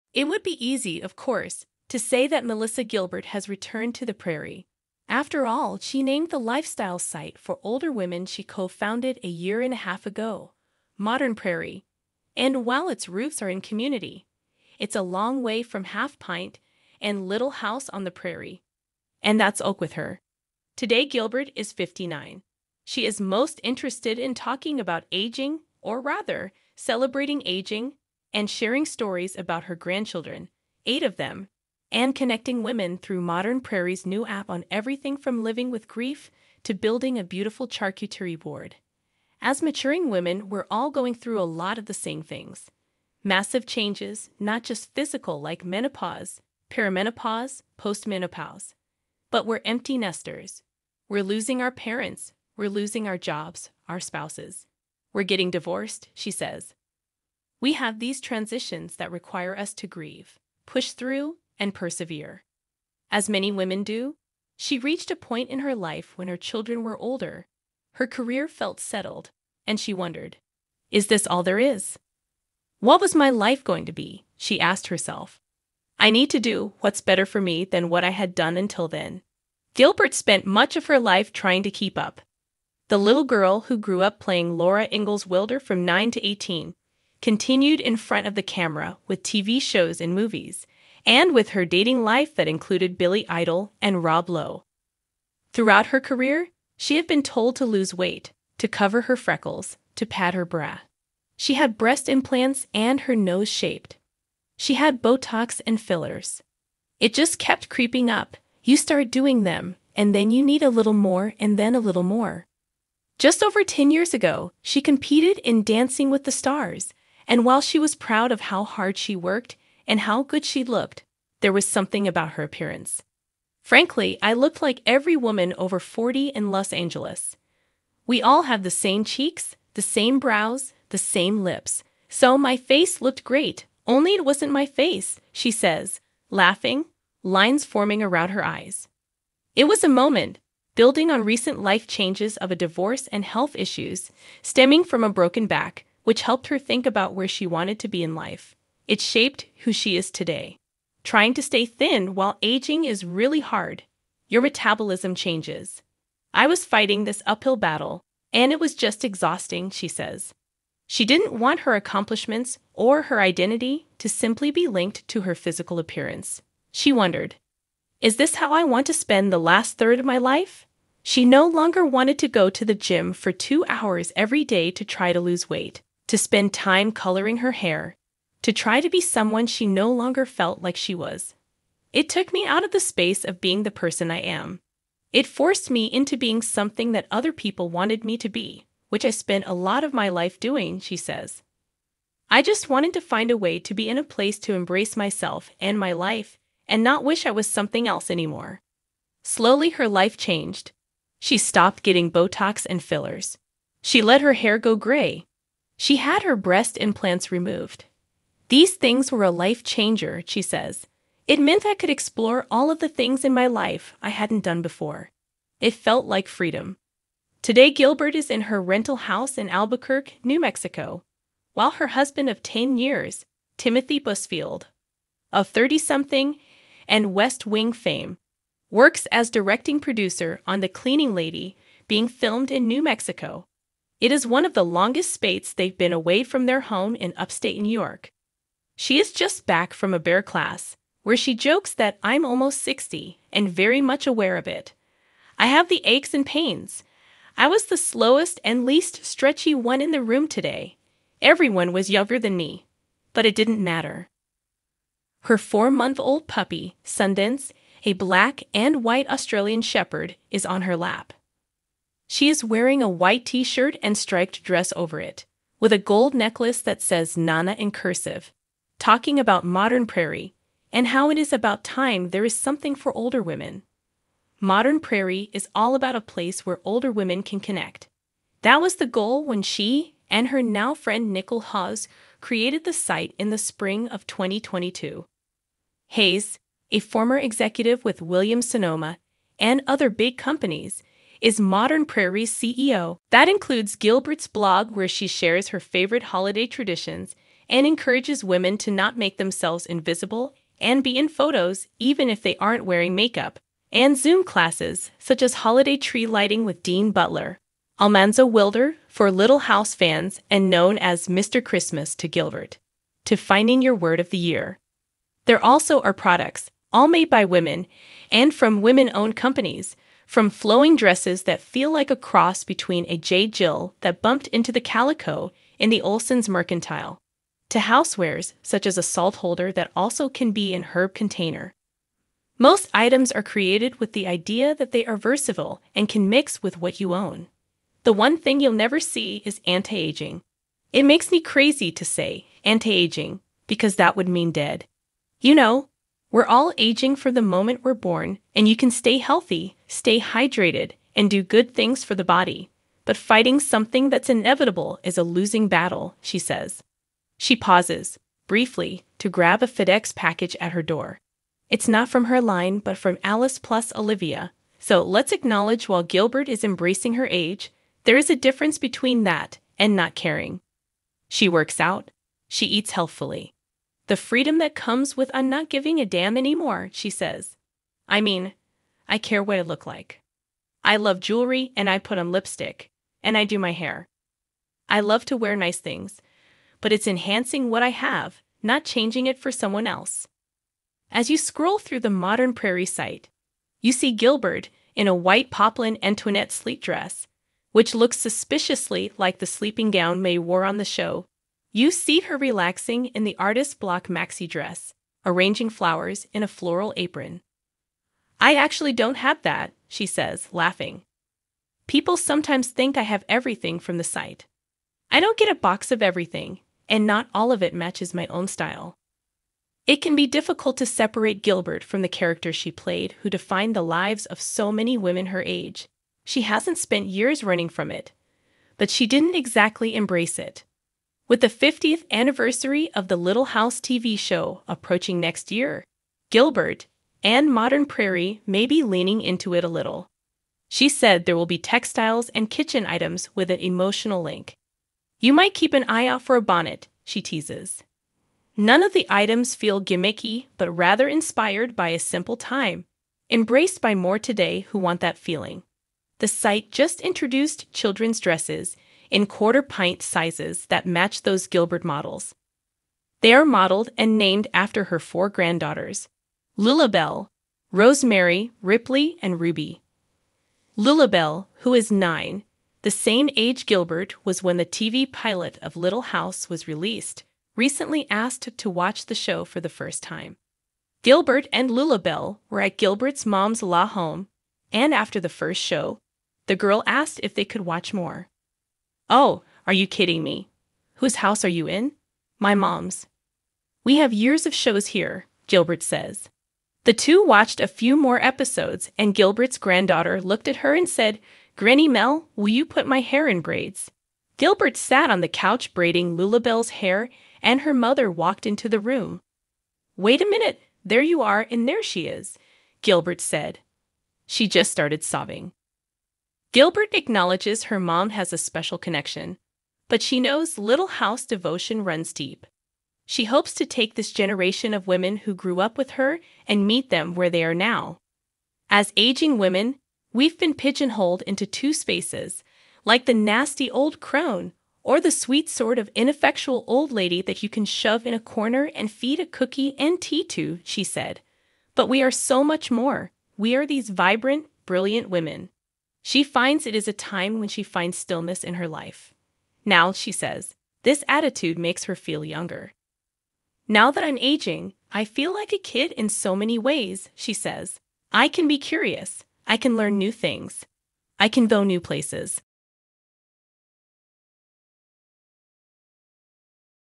OMG! 'Little House on the Prairie' star Melissa Gilbert Try Not To Gasp When You See Her Today
In a world often fixated on youth and beauty, Melissa Gilbert emerges as a beacon of authenticity, resilience, and reinvention. Best known for her iconic role as Laura Ingalls Wilder in "Little House on the Prairie," Gilbert has embarked on a new journey—one that celebrates the beauty of aging and embraces the complexities of life's ever-changing chapters.

At 59, Gilbert stands at the helm of Modern Prairie, a lifestyle platform designed to empower older women to navigate life's transitions with grace and confidence. Far from the prairie landscapes of her childhood fame, Modern Prairie is a digital haven where women gather to share stories, find support, and rediscover their passions in the golden years.

But Gilbert's journey goes beyond the glossy screens and Hollywood lights. It's a deeply personal exploration of self-discovery and acceptance, marked by pivotal moments of reflection and transformation. From the pressures of conforming to societal standards to the liberating decision to embrace her natural self, Gilbert's evolution embodies the essence of empowerment in every sense.

As she navigates the winding roads of life from her rental home in Albuquerque, New Mexico, Gilbert's presence radiates a sense of newfound freedom and authenticity. With her husband Timothy Busfield by her side, she embarks on a journey of self-exploration, unencumbered by the weight of expectations and the relentless pursuit of youth.

Through Modern Prairie, Gilbert extends a hand of solidarity to women everywhere, inviting them to embrace their unique journeys and celebrate the richness of experience that comes with age. From heartfelt blog posts to curated Zoom classes, Modern Prairie is a testament to the power of connection and community in the digital age.

As Gilbert delves into the next chapter of her life, she does so with unwavering grace and determination, inspiring women of all ages to embrace their true selves and find beauty in every stage of life. For Melissa Gilbert, the prairie may be a distant memory, but her legacy as a champion of authenticity and empowerment shines brighter than ever before.